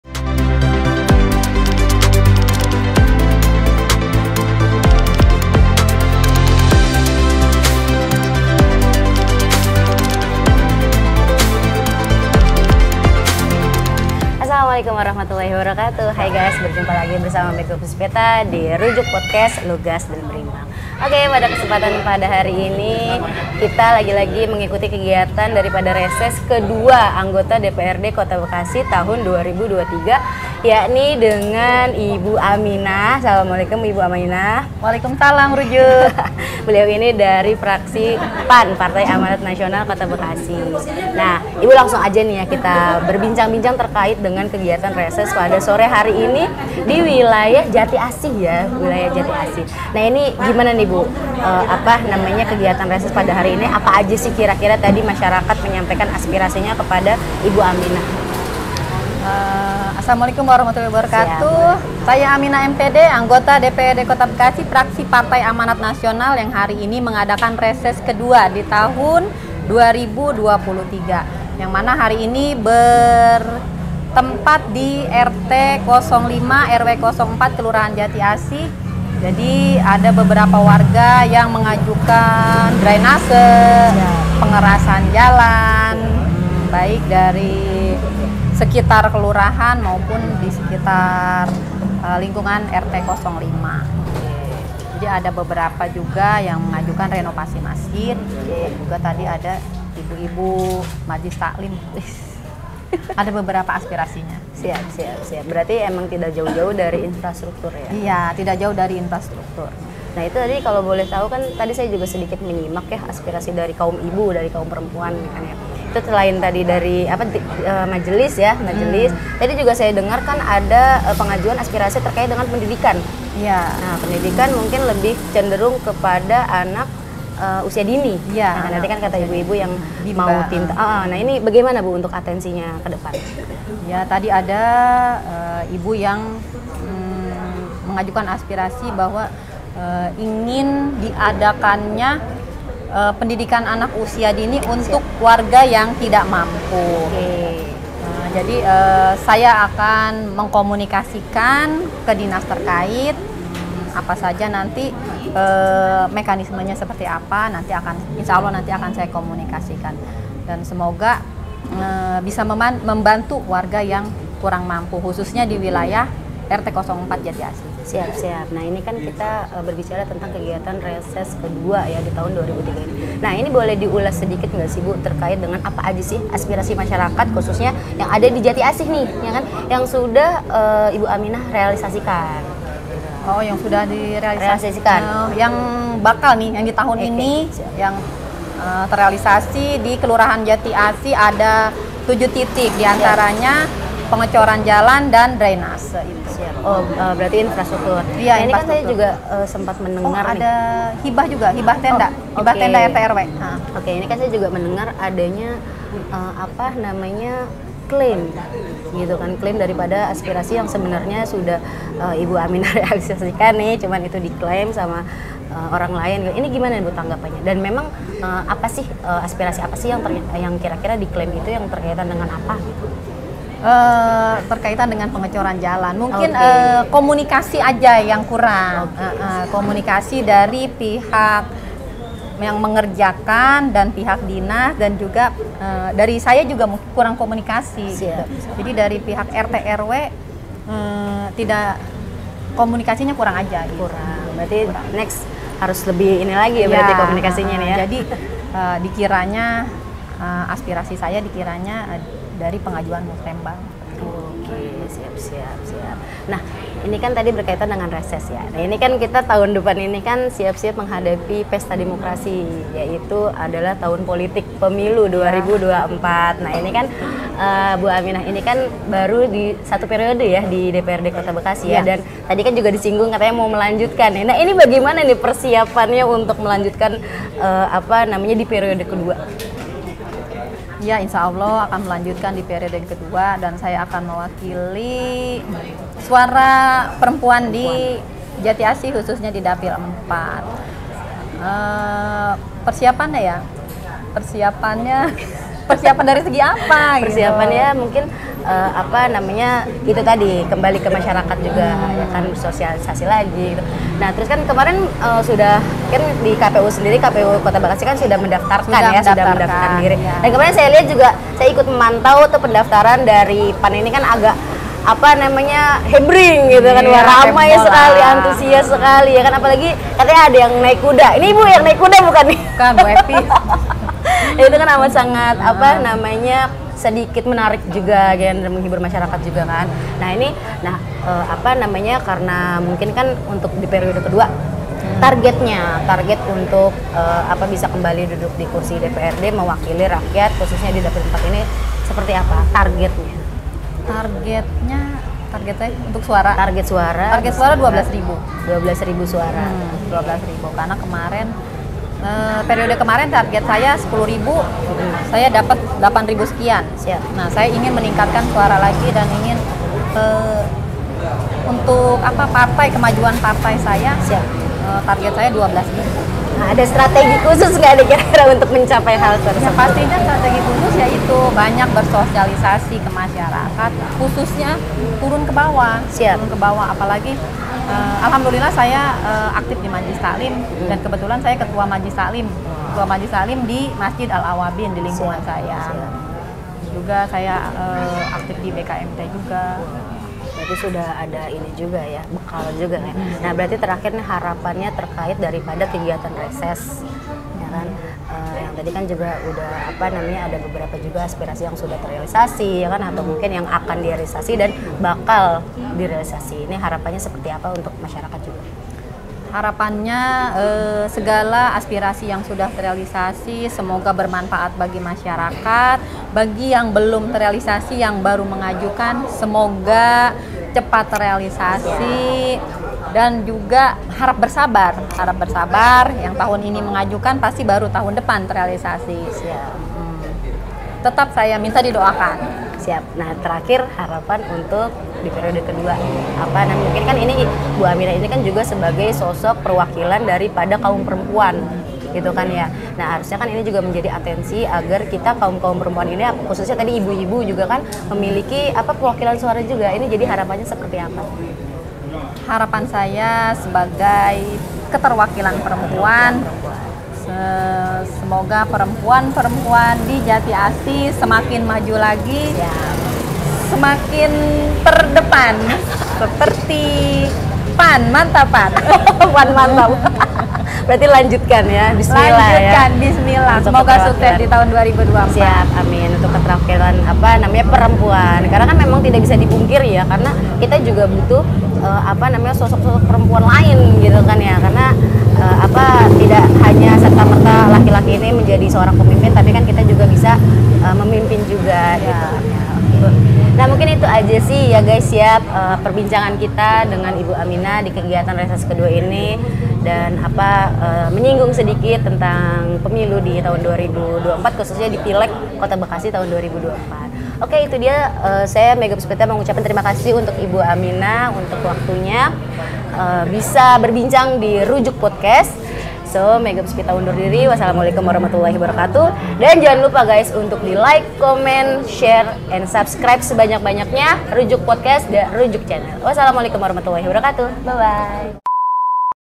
Reses II Tahun 2023, Aminah Serap Aspirasi Warga Jatiasih
Anggota DPRD Kota Bekasi dari Fraksi Partai Amanat Nasional (PAN), Aminah menggelar Jaring Aspirasi (Reses) ke-II Tahun 2023 di lingkungan RW 06, Kelurahan Jatiasih, Kecamatan Jatiasih, Sabtu (20/5/2023) sore.